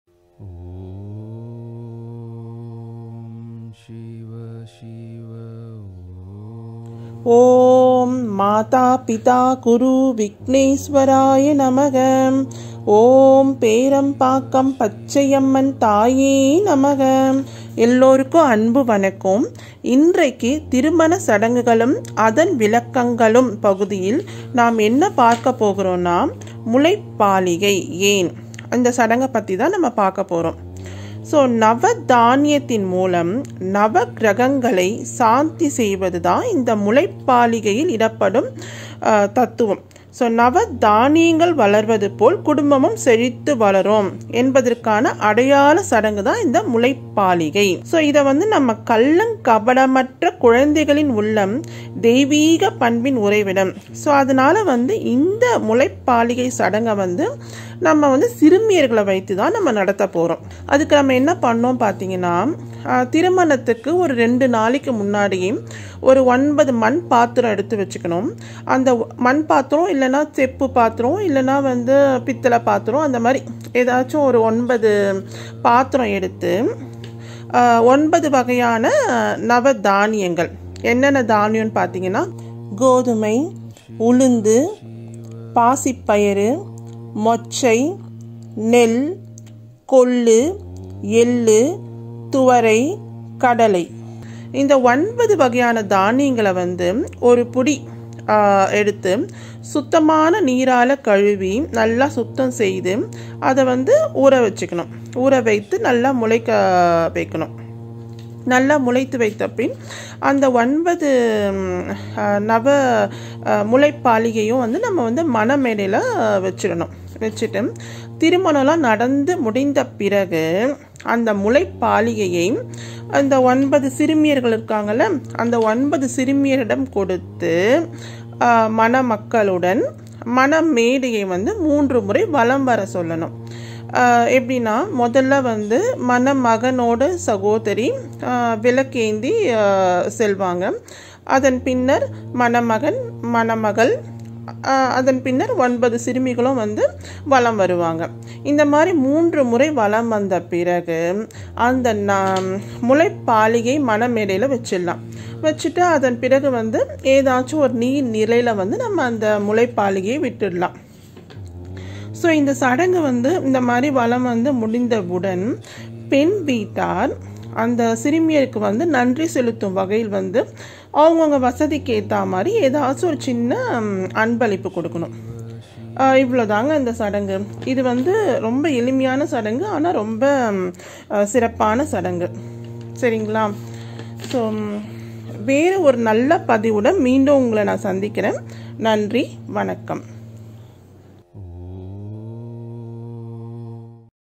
01 - திருமண சடங்கு - முளைப்பாலிகை ஏன்
திருமண சடங்குகளும் அதன் விளக்கமும்- 1 முளைப்பாலிகை ஏன்

Marriage Ritual Practice steps

நவதானியங்கள்

கோதுமை,
உளுந்து,
பாசிப்பயறு,
மொச்சை
நெல்
கொள்ளு,,
எள்ளு
துவரை,
கடலை